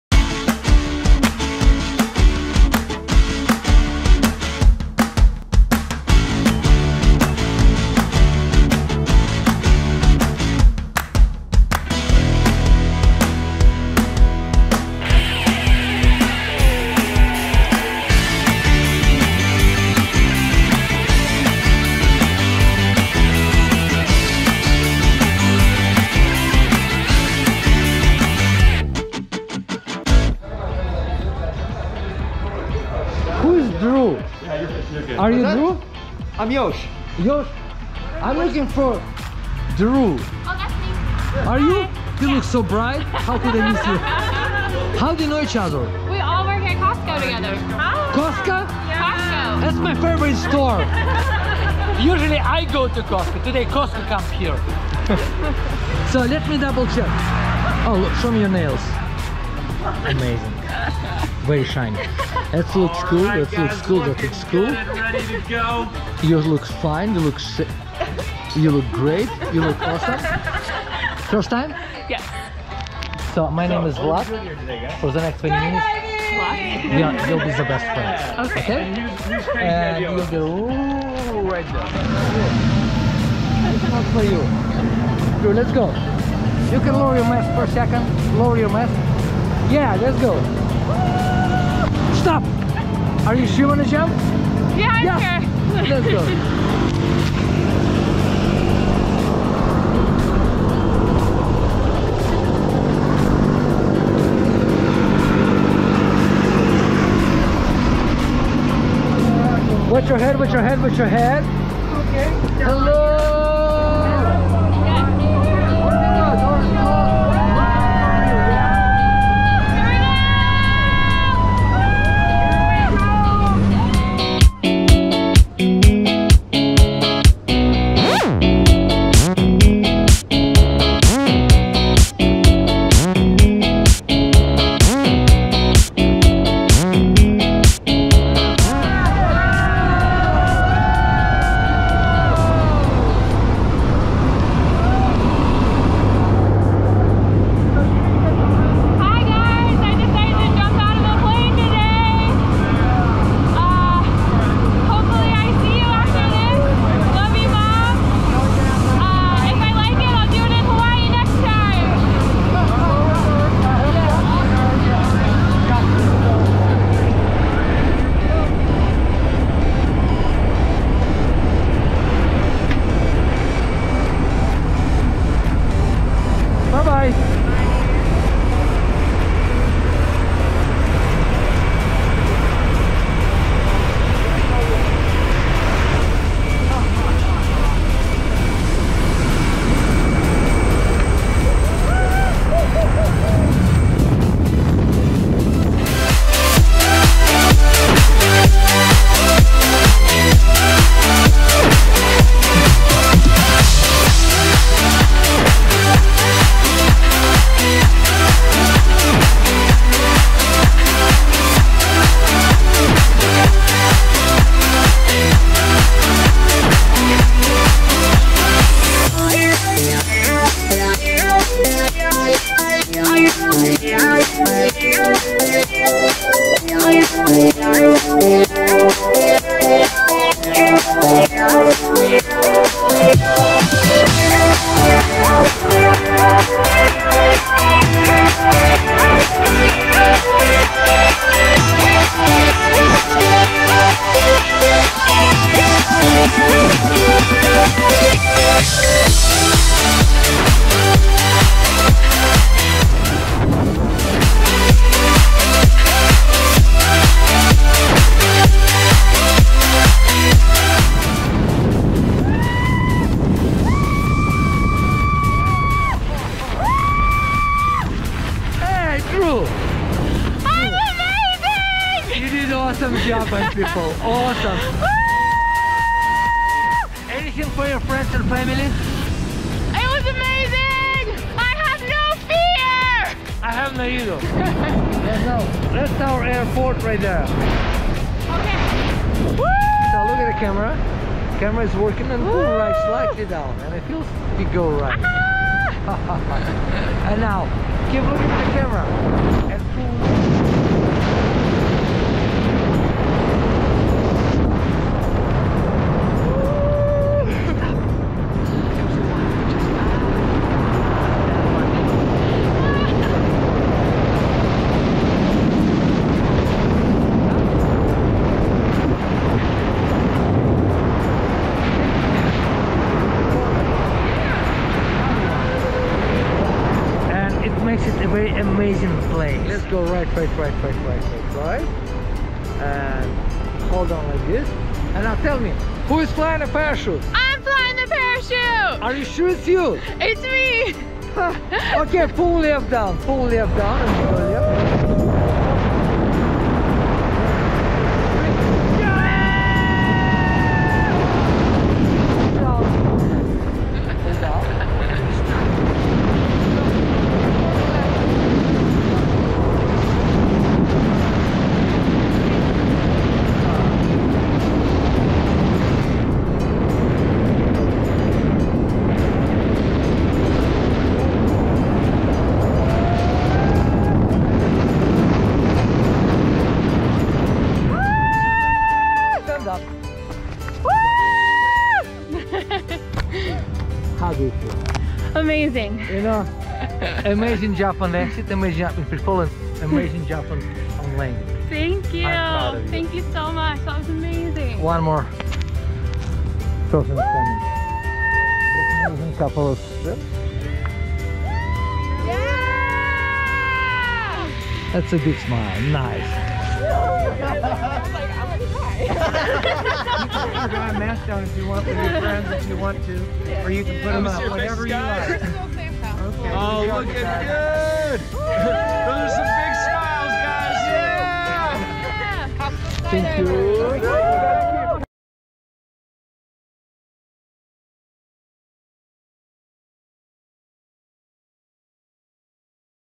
Drrue Machia's Tandem skydive!
Drrue Machia's tandem skydive at Skydive Snohomish!